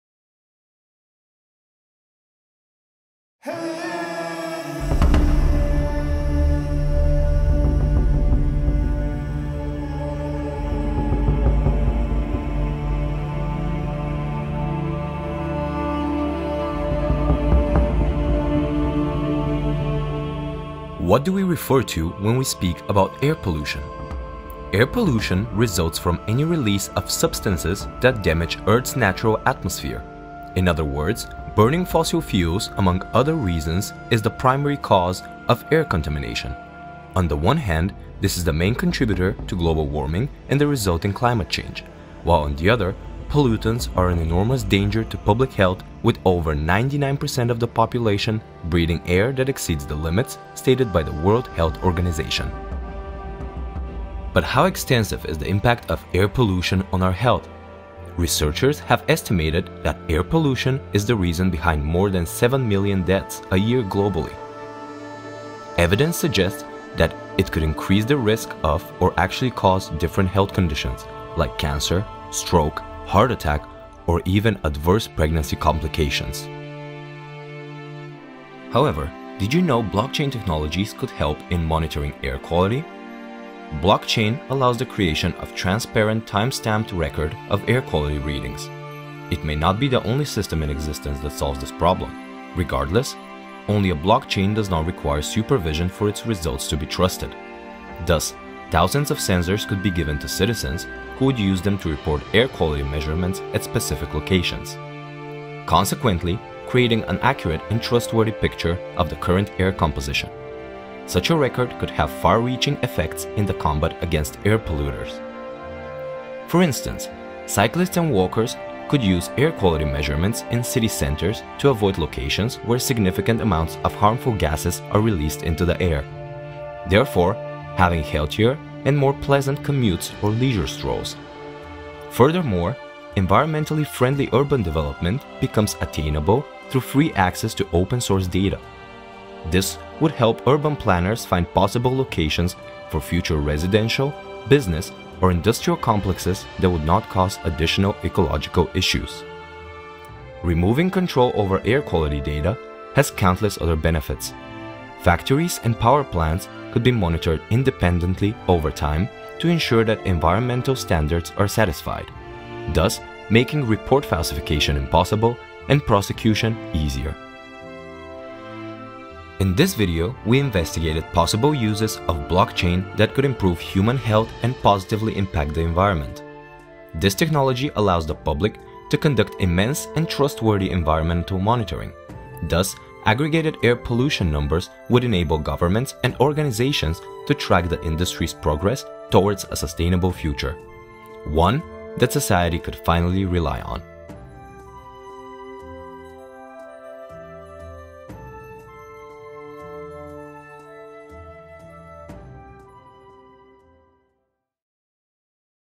BLAST - Blockchain Applications for Air Pollution
Uncover the potential of blockchain technology in monitoring air quality and fighting air pollution. This video is a part of the BLAST! series, created under the BLAST! Project (co-funded by the European Commision) which investigates the potential of blockchain technology in addressing environmental challenges.

In this video, we explore how blockchain can provide a transparent, time-stamped record of air quality readings, leading to improved environmental monitoring and public health. Learn how the use of blockchain can empower citizens, urban planners, and governments to make informed decisions, promote environmentally friendly urban development, and ensure industries adhere to environmental standards. Join us in examining how blockchain technology could pave the way for a healthier, more sustainable future.